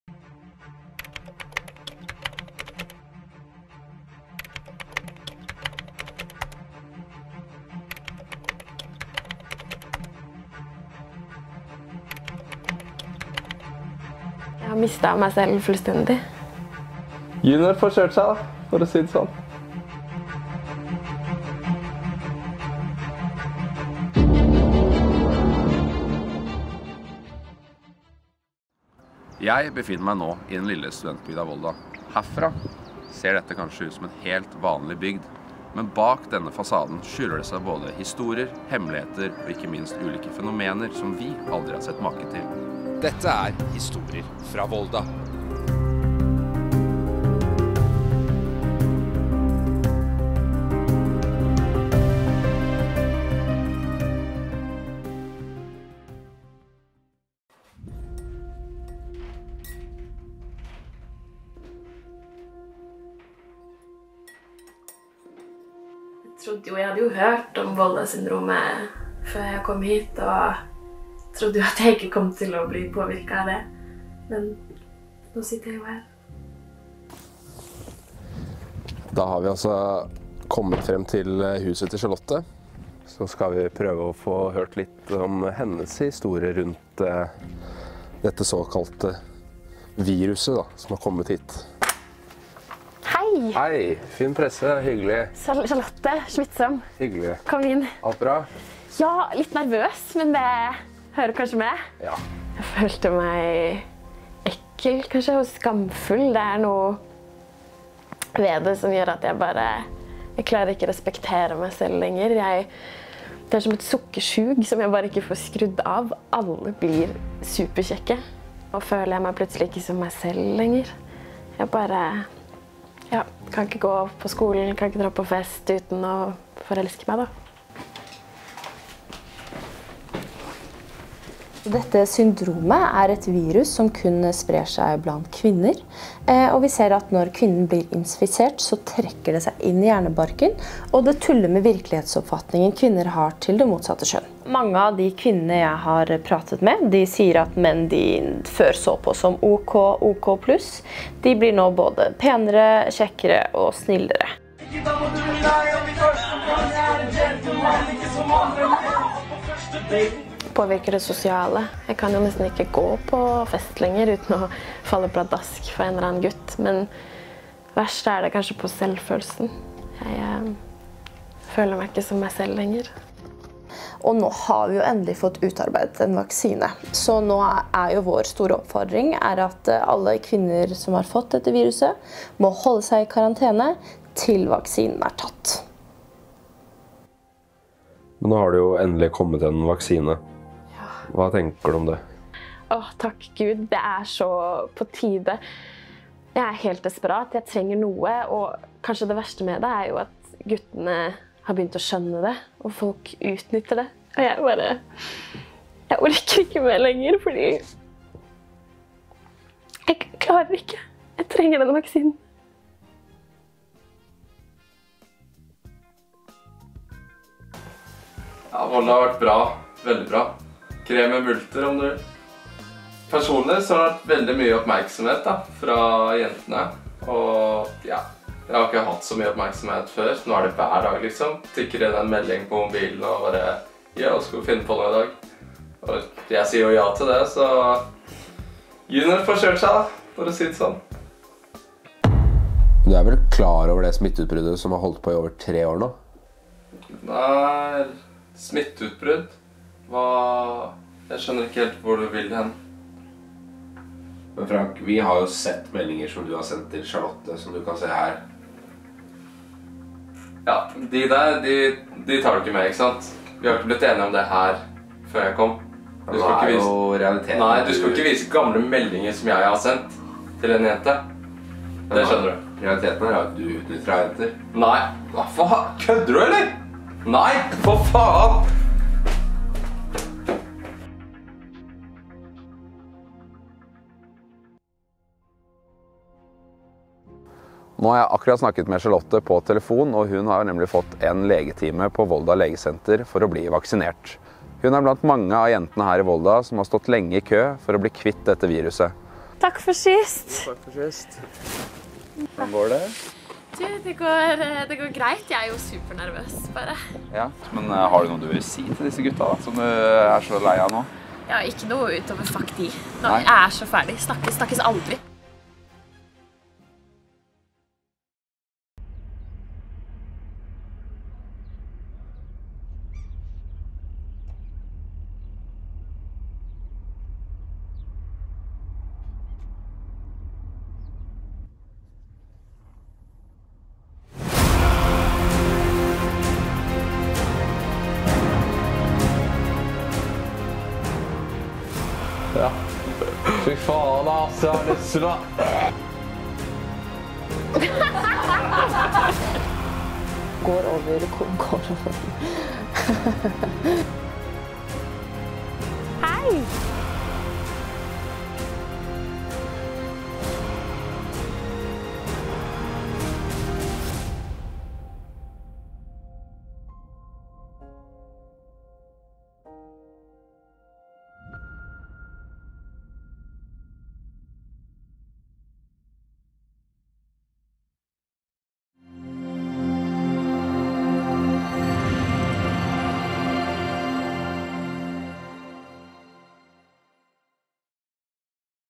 Historier fra Volda — Voldasyndromet
Voldasyndromet er et virus som har herjet i Volda i flere år. Journalist Finn Presse, har dratt til studentbygda for å få et nærmere innblikk i dette fenomenet.

Av og med:
Kristoffer Klepp Moe
Selma Høgås Røen
Henrik Hegg Johansen
Jørgen Eriksen
Henriette Nordheim
Johannes Havig-Gjelseth
Christina Engen Johansen
Louise Grøvan
Håvard Sjursen
Nicolai Wølner Voss